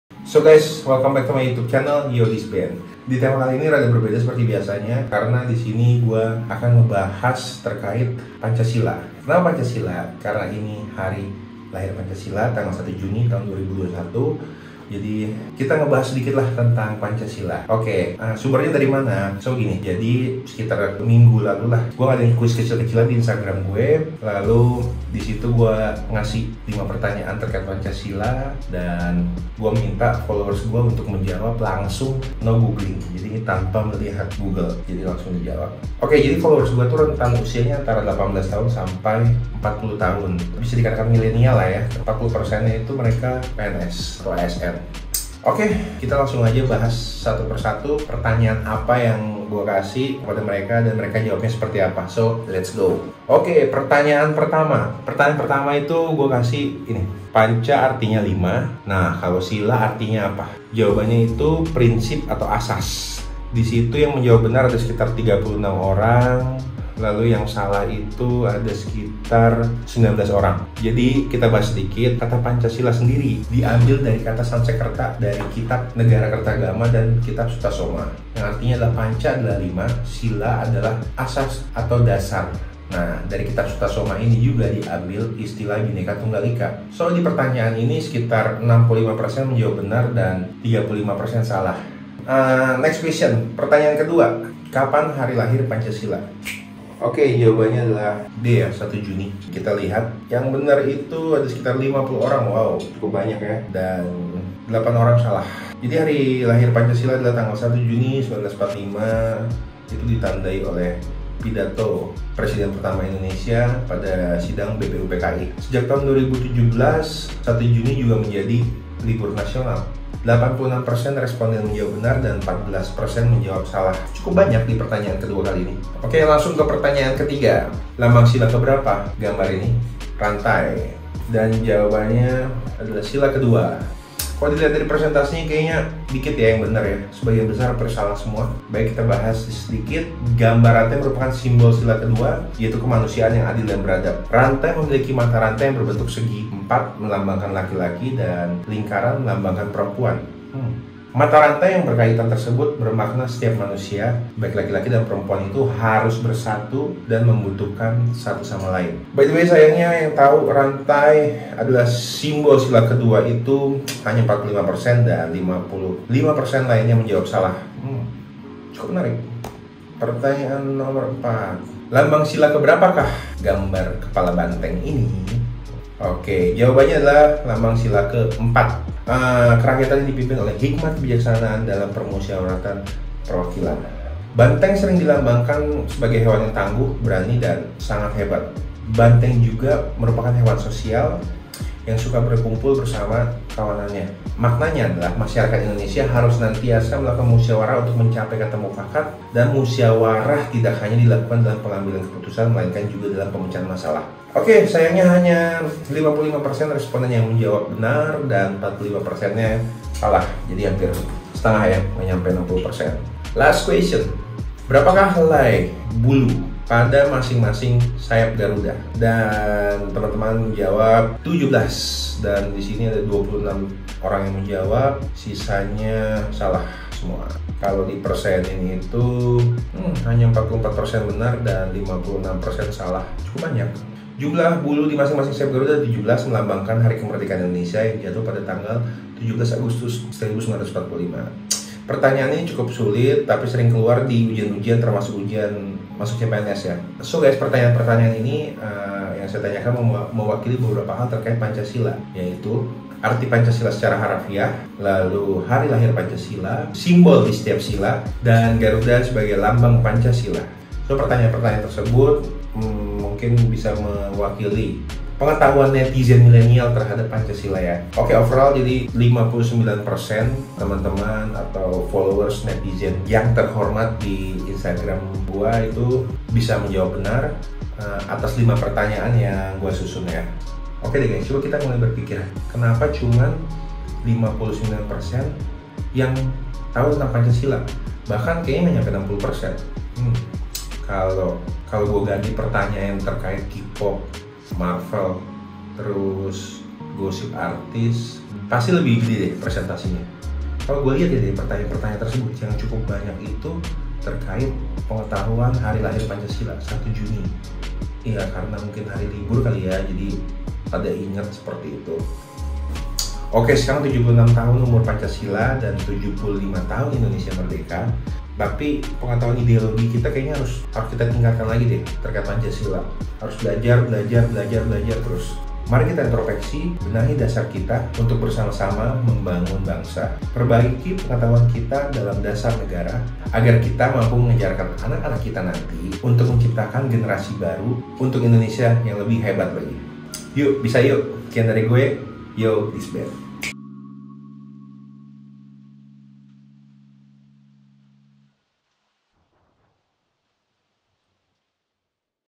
SEBERAPA TAU TENTANG PANCASILA⁉️
1 JUNI 2021 Hari Lahir Pancasila yang ke-76
Pada Video ini kita ngebahas beberapa pertanyaan tentang Pancasila yang minggu lalu ditanyakan di IG, kita lihat aja langsung yuk...

Produced by : Benhard Okta Sabathian
Director : Benhard Okta Sabathian
Scriptwriter : BOSP x CSR